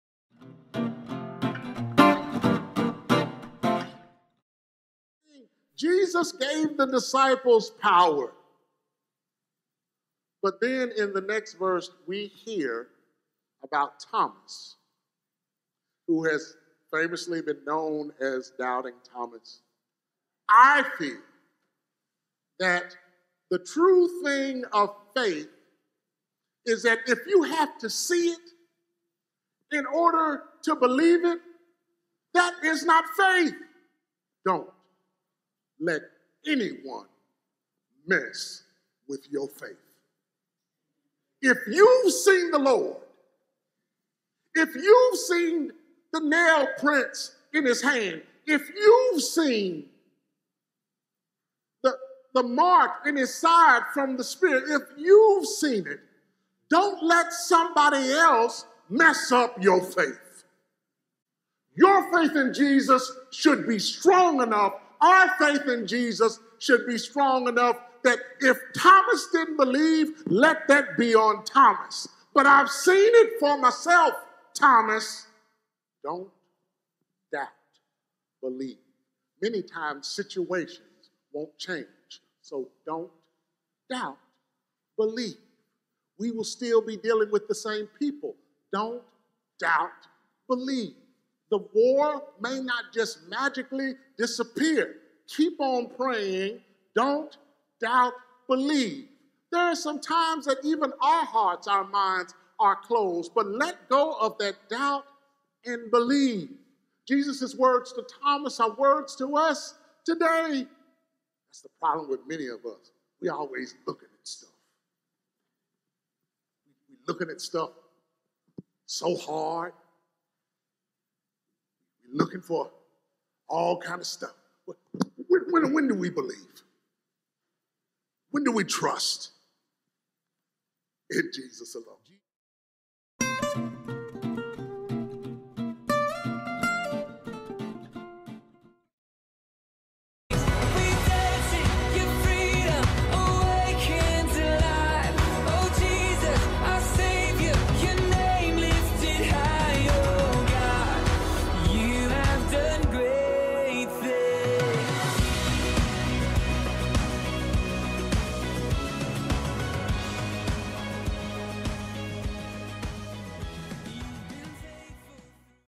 Sermon Shorts l Peace Be With You
Enjoy this week's Sermon Shorts l Peace be with You. To view the entire service or find out more about Highland Valley go to hvumc.org.